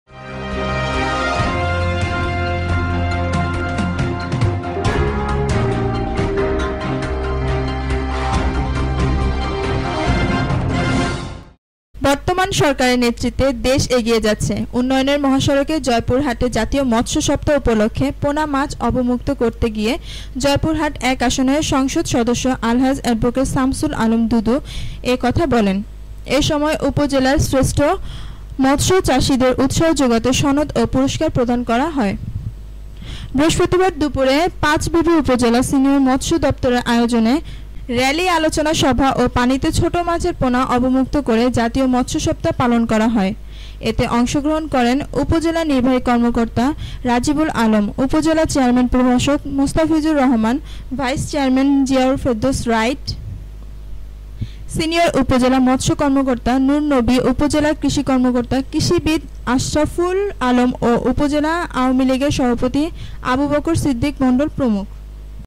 জয়পুরহাটে জাতীয় মৎস সপ্তাহ উপলক্ষ্যে মাছের পোনা অবমুক্ত করন|Joypurhat|71Bangla TV
"বর্তমান সরকারের নেতৃত্বে দেশ এগিয়ে যাচ্ছে উন্নয়নের মহাসরকে"জয়পুরহাটে জাতীয় মৎস সপ্তাহ উপলক্ষ্যে পোনা মাছ অবমুক্ত করতে গিয়ে জয়পুরহাট-১আসনের সংসদ সদস্য আলহাজ্ব এ্যাডঃসামছুল আলম দুদু(এমপি)এ কথা বলেন।এসময় উপজেলার শ্রেষ্ঠ মৎস চাষীদের উৎসাহ যোগাতে সনদ ও পুরস্কার প্রদান করেন হয়।
বৃহস্পতিবার দুপুরে,পাঁচবিবি উপজেলা সিনিয়র মৎস দপ্তরের আয়োজনে র্যালী,আলোচনা সভা ও পানিতে ছোট মাছের পোনা অবমুক্ত করে জাতীয় মৎস সপ্তাহ্ পালন করা হয়।
এতে অংশগ্রহন করেন,
 উপজেলা নির্বাহী কর্মকর্তা,রাজিবুল আলম,উপজেলা চেয়ারম্যান প্রভাষক মোস্তাফিজুর রহমান,ভাইস-চেয়ারম্যান জিয়াউল ফেরদৌস রাইট, সিনিয়র উপজেলা মৎস কর্মকর্তা নূর-নবী, উপজেলা কৃষি কর্মকর্তা কৃষিবিদ আশরাফুল আলম ও উপজেলা আঃলীগের সভাপতি আবু বকর সিদ্দিক মন্ডল প্রমুখ।